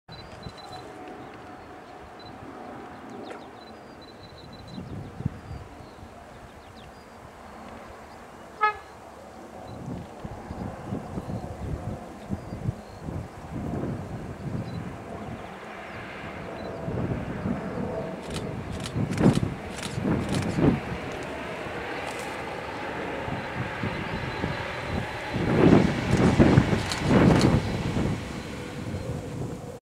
37682 & 37194 Cross Eynsford Viaduct
37682 & 37194 Cross Eynsford Viaduct Working 6M95 Dungeness - Willesden

Sorry about the funny angle at the end